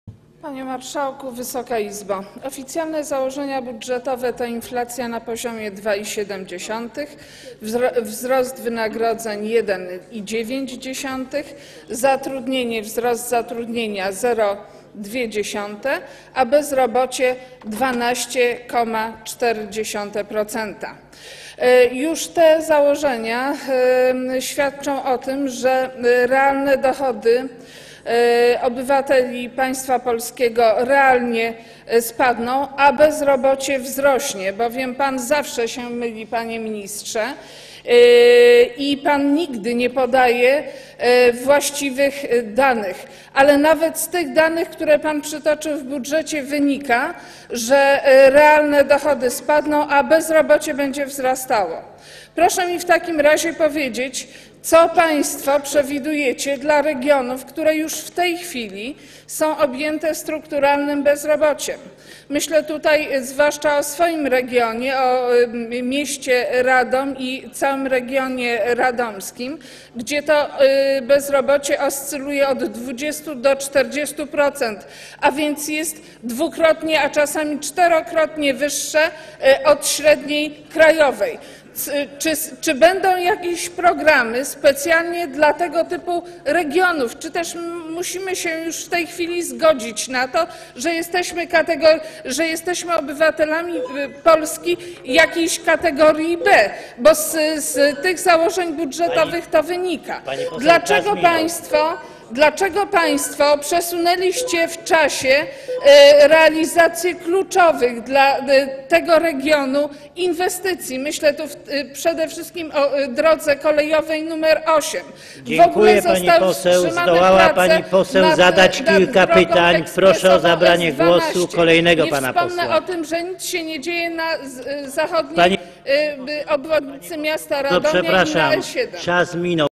[128/401] Marzena Dorota Wróbel: Oﬁcjalne założenia budżetowe to: inﬂacja na poziomie 2,7...
Posiedzenie Sejmu nr 24
23 października 2012 r. 15:55:27
Poseł Marzena Dorota Wróbel:

Panie Marszałku!  Wysoka Izbo!  Oﬁcjalne założenia budżetowe to: inﬂacja na poziomie 2,7%, wzrost wynagrodzeń -- 1,9%, wzrost zatrudnienia -- 0,2%, a wskaźnik bezrobocia -- 12,4%.  Już te założenia świadczą o tym, że realne dochody obywateli państwa polskiego spadną, a bezrobocie wzrośnie, bowiem pan zawsze się myli, panie ministrze, i pan nigdy nie podaje właściwych danych.  Ale nawet z tych danych zawartych w budżecie, które pan przytoczył, wynika, że realne dochody spadną, a bezrobocie będzie wzrastało.

Proszę w takim razie powiedzieć, co państwo przewidujecie dla regionów, które już w tej chwili są objęte strukturalnym bezrobociem.  Myślę zwłaszcza o swoim regionie, o mieście Radom i całym regionie radomskim, gdzie bezrobocie oscyluje w granicach od 20% do 40%, a więc jest dwukrotnie, a czasami czterokrotnie, wyższe od średniej krajowej.  Czy będą jakieś programy specjalnie dla tego typu regionów, czy też musimy już w tej chwili zgodzić się na to, że jesteśmy obywatelami Polski kategorii B?

(Dzwonek)

Bo z tych założeń budżetowych to wynika.

Wicemarszałek Eugeniusz Tomasz Grzeszczak: Pani poseł, czas minął.

Dlaczego państwo przesunęliście w czasie realizację kluczowych dla tego regionu inwestycji?  Myślę przede wszystkim o drodze kolejowej nr 8.

Wicemarszałek Eugeniusz Tomasz Grzeszczak: Dziękuję, pani poseł.

W ogóle zostały wstrzymane prace nad drogą ekspresową S12.

Wicemarszałek Eugeniusz Tomasz Grzeszczak: Zdołała pani poseł zadać kilka pytań. Proszę o zabranie głosu kolejnego pana posła.

Nie wspomnę o tym, że nic nie dzieje się na zachodniej obwodnicy miasta Radomia i na E7.

(Oklaski)